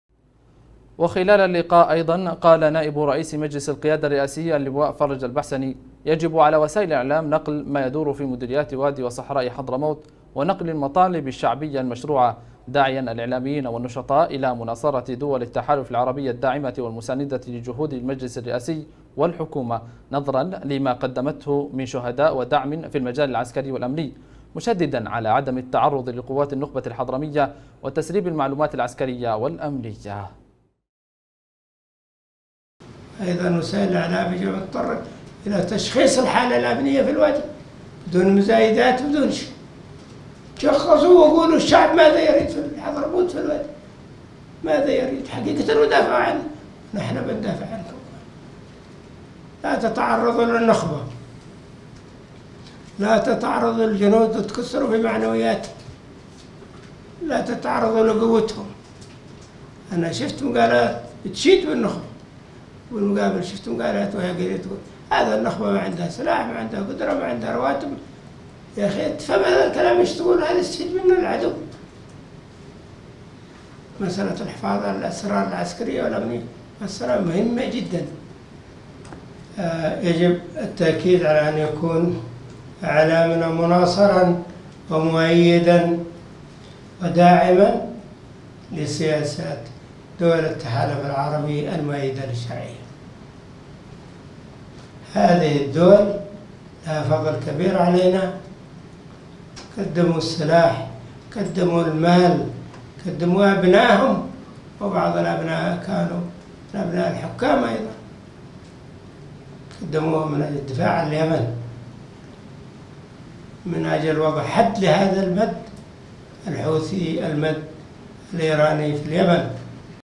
خبر | اللواء البحسني يدعو الإعلاميين والنشطاء إلى نقل مطالب أبناء وادي حضرموت المشروعة
قال نائب رئيس مجلس القيادة الرئاسي اللواء فرج البحسني يجب على وسائل الإعلام نقل ما يدور في مديريات وادي وصحراء حضرموت ونقل المطالب الشعبية المشروعة داعياً الإعلاميين والنشطاء إلى مناصرة دول التحالف العربي الداعمة والمساندة لجهود المجلس الرئاسي والحكومة نظرًا لما قدمته من شهداء ودعم في المجال العسكري والأمني مشدداً على عدم التعرض لقوات النخبة الحضرمية وتسريب المعلومات العسكرية والأمنية

تابعنا على شبكات التواصل الإجتماعي:
ــــــــــــــــــــــــــــــــــــــــــــــــــــــــــ